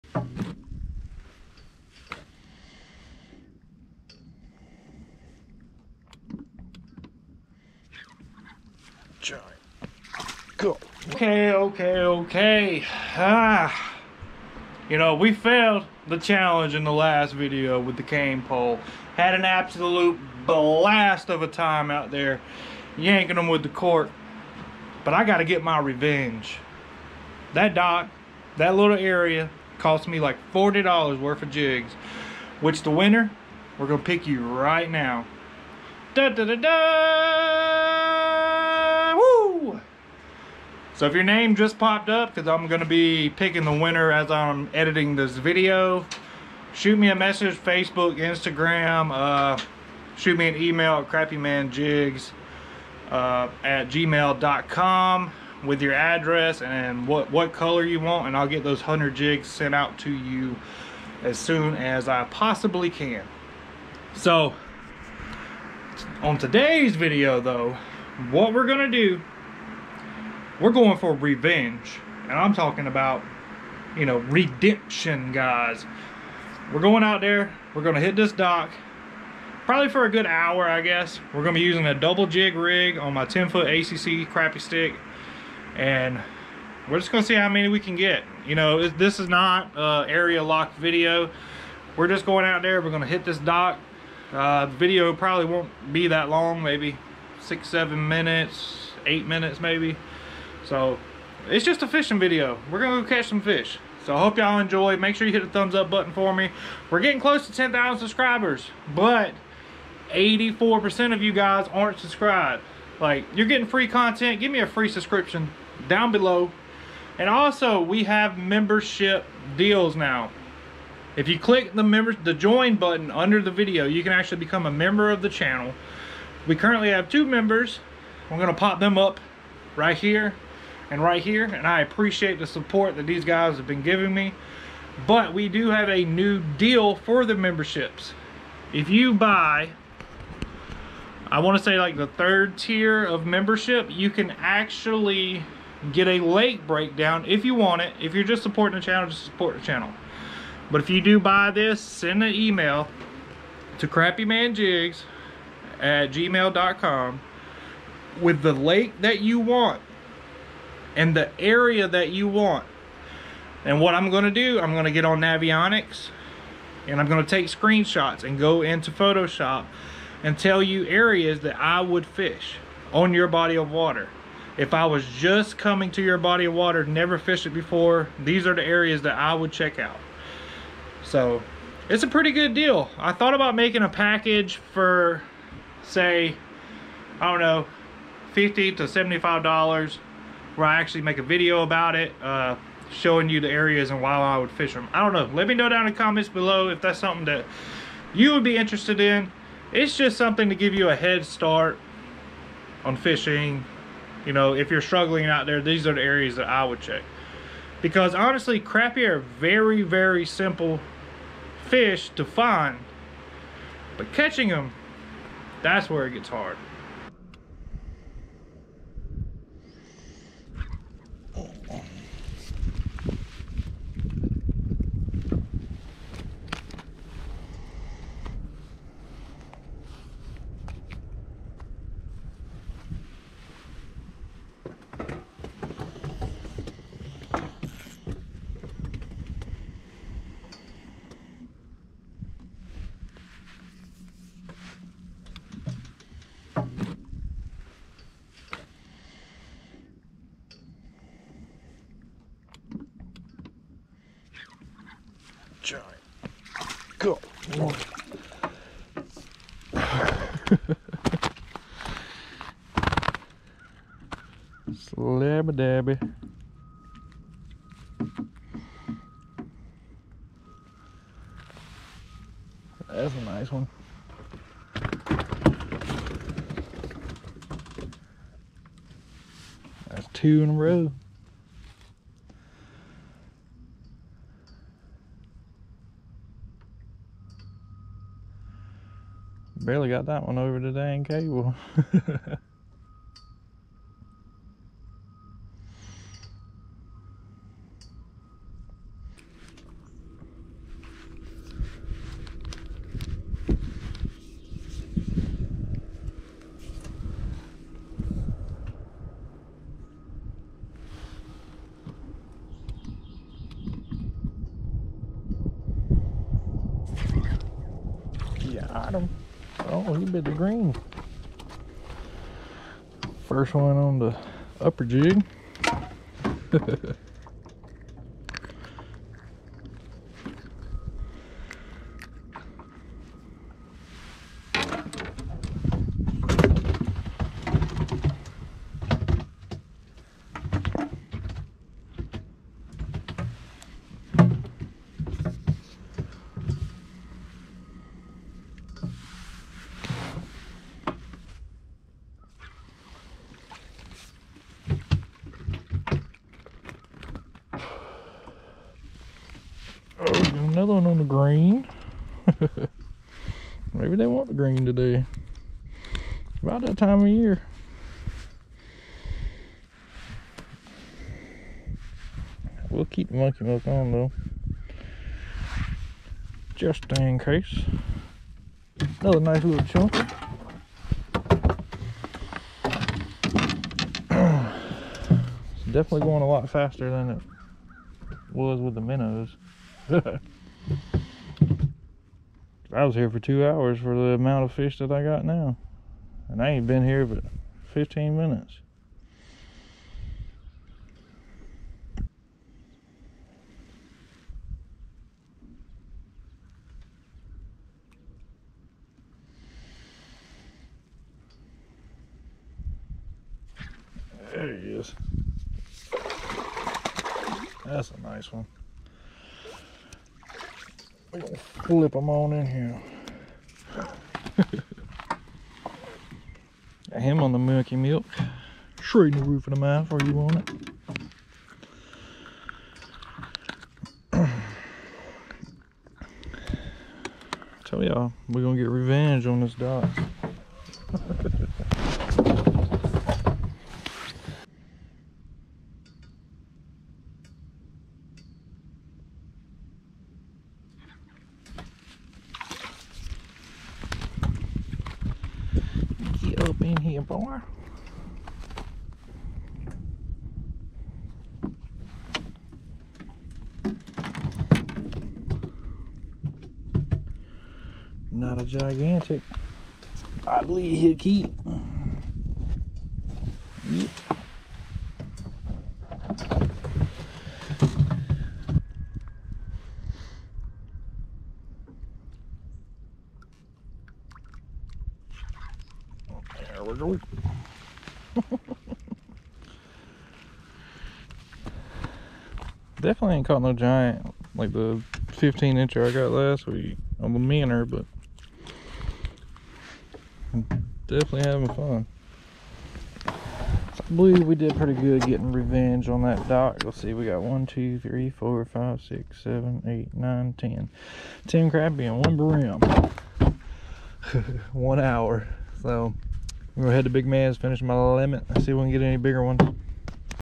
I Had To Hit This Dock AGAIN | Crappie Fishing 2022
Went back to get the slabs off this dock.